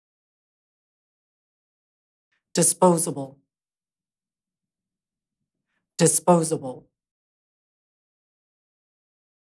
How to pronounce DISPOSABLE in American English
This video shows you how to pronounce DISPOSABLE in American English. Speaker has an accent from Ames, Iowa. Collins Dictionary, the home of living English and pioneers of dictionary publishing